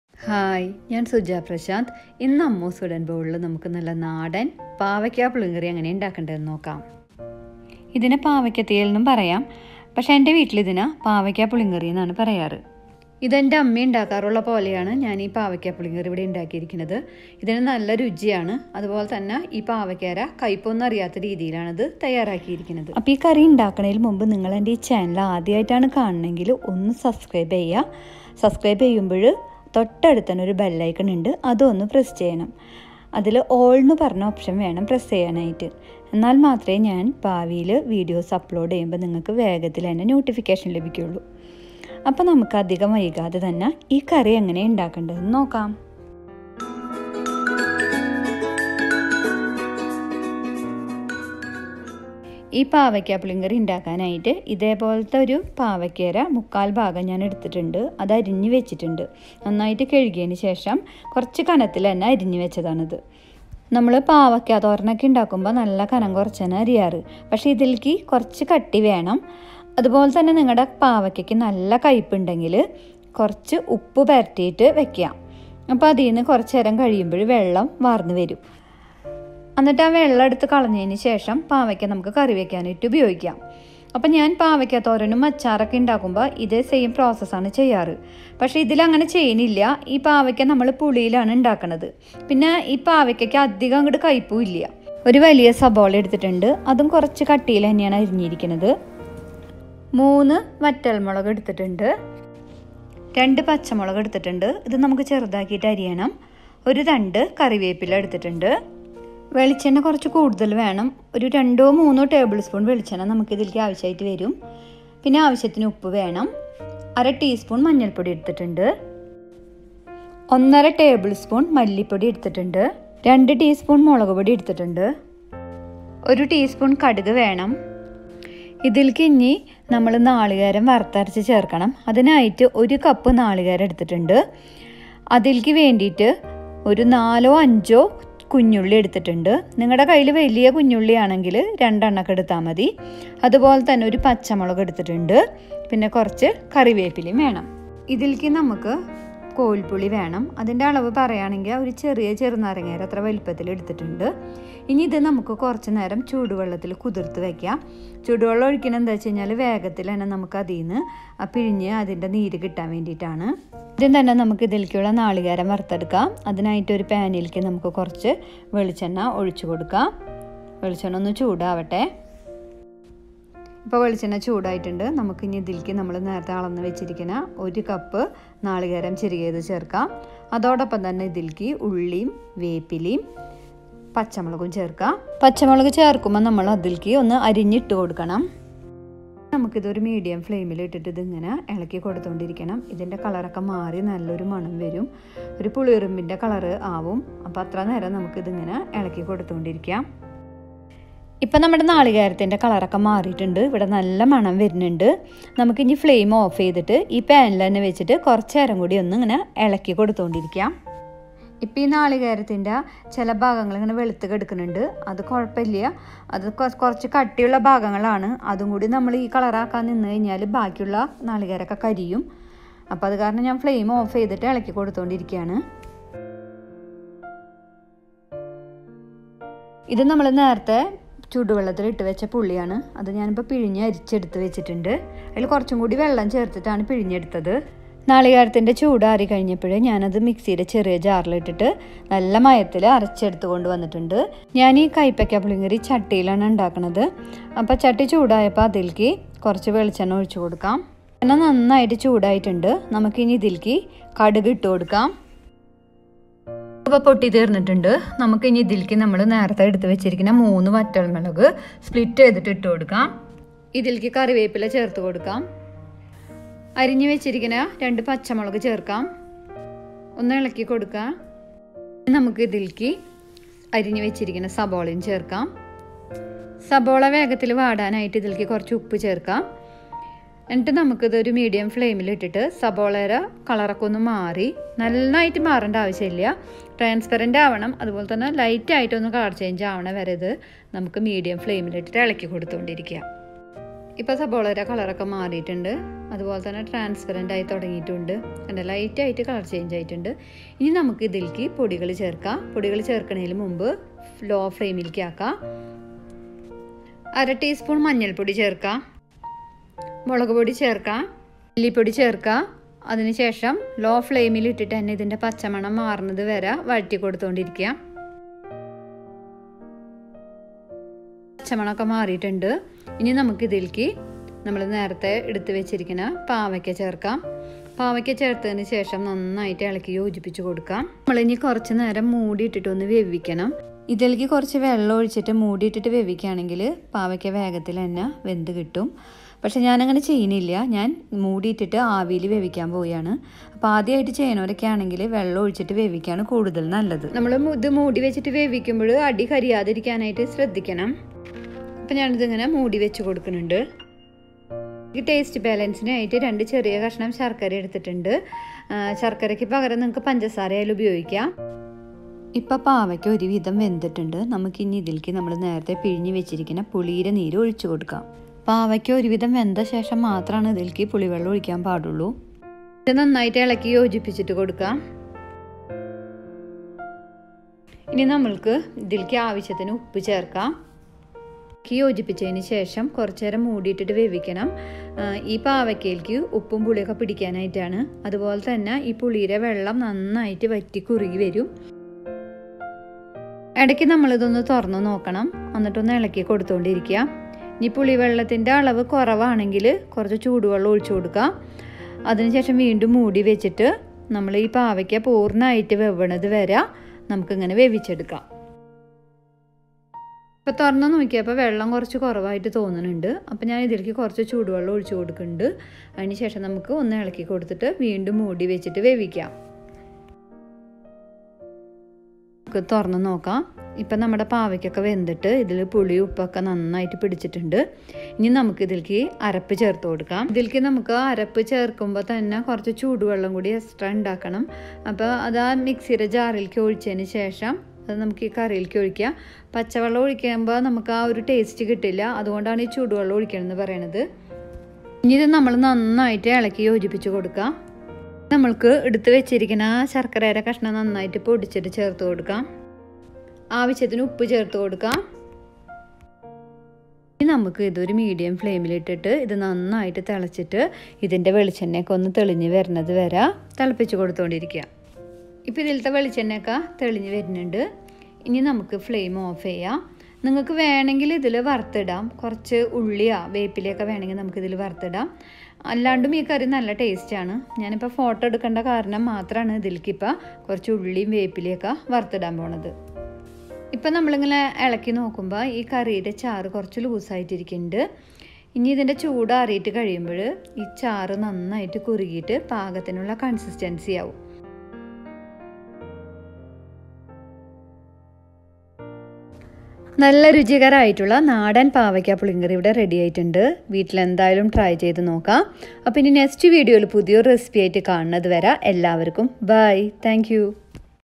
കയ്പില്ലാത്ത പാവയ്ക്കകറി|| Pavakka Theeyal || Pavakka Varutharacha Curry
Namasthae. I'm Suja Prasanth... this video is about how to make tasty Pavakka varutharacha curry/ Pavakka Theeyal). It is one of the famous authentic Kerala recipe.In my childhood days my mother usually makes this bitterless bitter gourd curry .